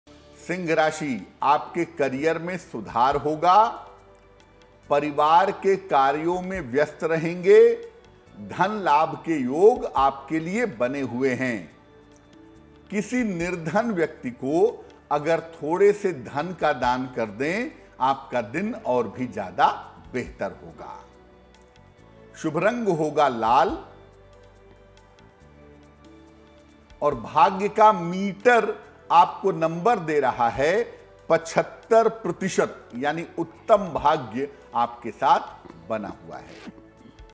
Aaj Ka Singh Rashifal 5 दिसंबर 2022: धन लाभ के योग हैं | Leo Horoscope Today
Aaj Ka Rashifal Singh Rashi 5 दिसंबर 2022: सिंह राशि के जातकों को  धन लाभ के योग हैं. करियर में सुधार होगा. परिवार के कार्यों में व्यस्त रहेंगे.धन का दान करें. आज शुभ रंग लाल है.